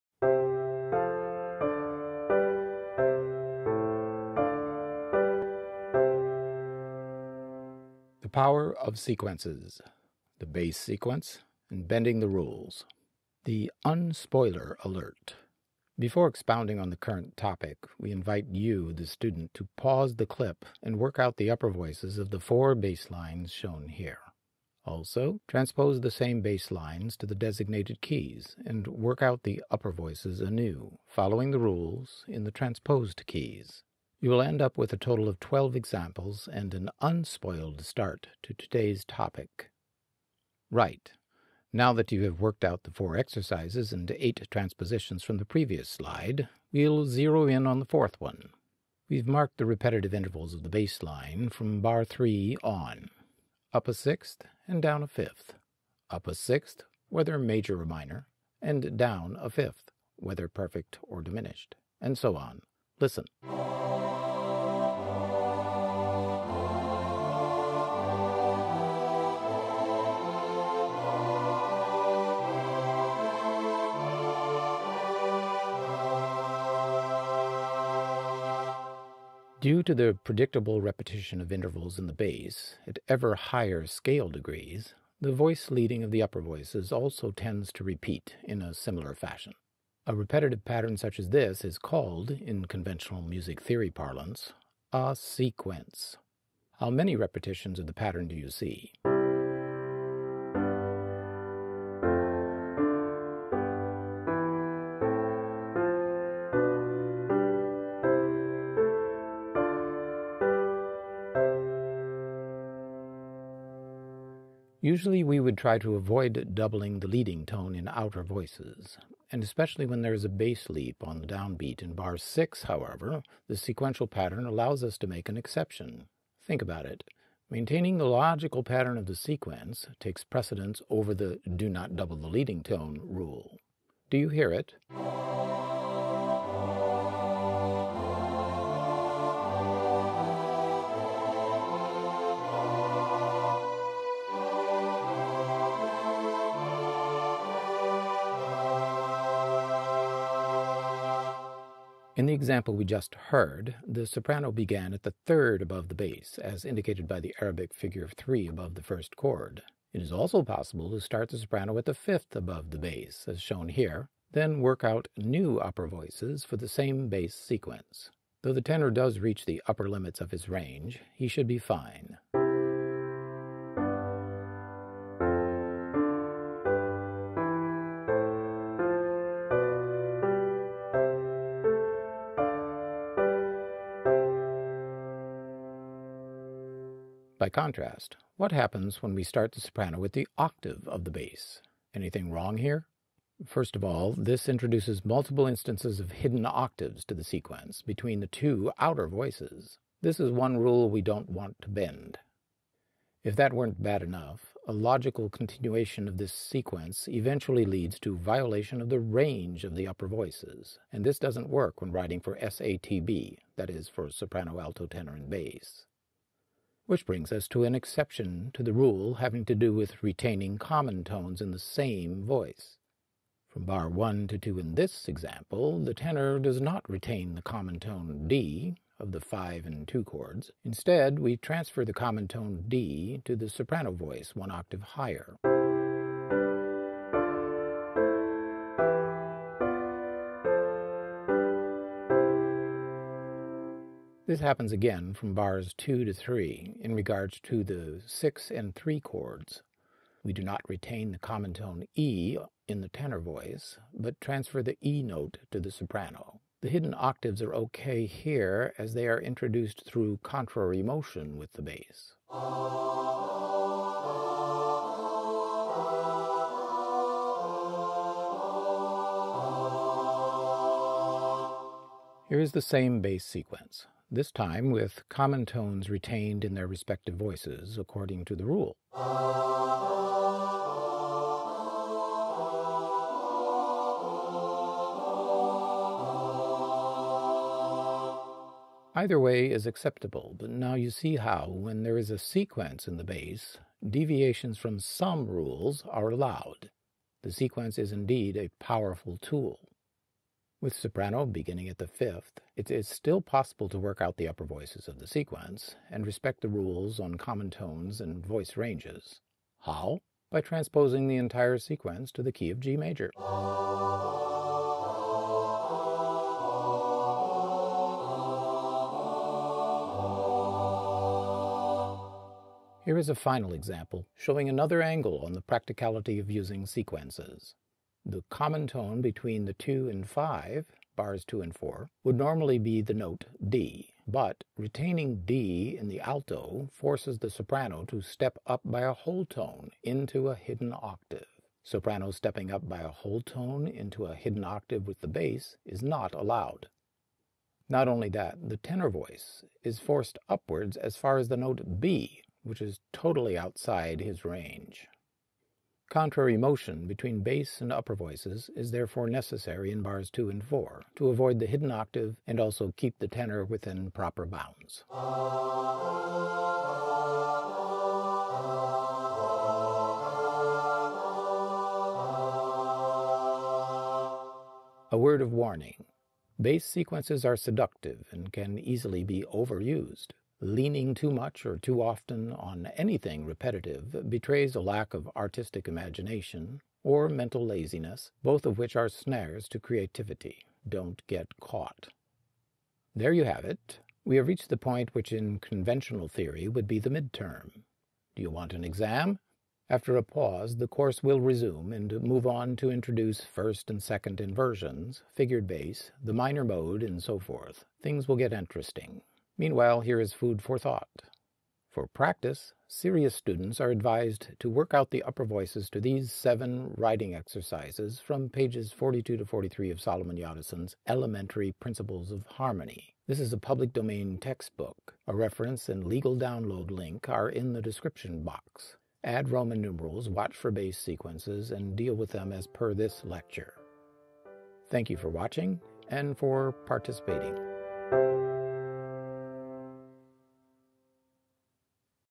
The Power of Sequences - Harmony I, #17
The Power of Sequences – The Bass Sequence, and Bending the Rules
The Un-Spoiler Alert: Before expounding on the current topic, we invite you, the student, to pause the clip and work out the upper voices of the four bass lines shown here. Also, transpose the same bass lines to the designated keys, and work out the upper voices anew, following the Rules, in the transposed keys. You will end up with a total of 12 examples, and an unspoiled start to today's topic.
Right. Now that you have worked out the four exercises and eight transpositions from the previous slide, we’ll zero-in on the fourth one. We’ve marked the repetitive intervals of the bass line, from bar 3 on: Up a 6th, and down a 5th. Up a 6th (whether major or minor), and down a 5th (whether perfect or diminished), and so on. Listen. [mus]
Due to the predictable repetition of intervals in the bass, at ever higher scale degrees, the voice leading of the upper voices also tends to repeat in a similar fashion. [69d + uppers; sequential bars labeled] A repetitive pattern such as this is called, in conventional music theory parlance, a Sequence. How many repetitions of the pattern do you see? [mus]
Usually we would try to avoid doubling the LT in outer voices, and especially when there is a bass leap, on the downbeat. In bar 6, however, the sequential pattern allows us to make an exception. Think about it! Maintaining the logical pattern of the Sequence takes precedence over the do-not-double-the-LT Rule. Do you hear it? (...)

Jadassohn, Dr. Salomon. Elementary Principles of Harmony for School and Self-Instruction, pp. 42-43. Breitkopf and Hartel, 1895. A Public Domain work.

________. Manual of Harmony, pp. 25-29. Breitkopf and Hartel, 1890. A Public Domain work.

Internet Archive Link – must copy url and paste into browser – for download of [Elementary Principles]

Internet Archive Link – must copy url and paste into browser – for download of [Harmony]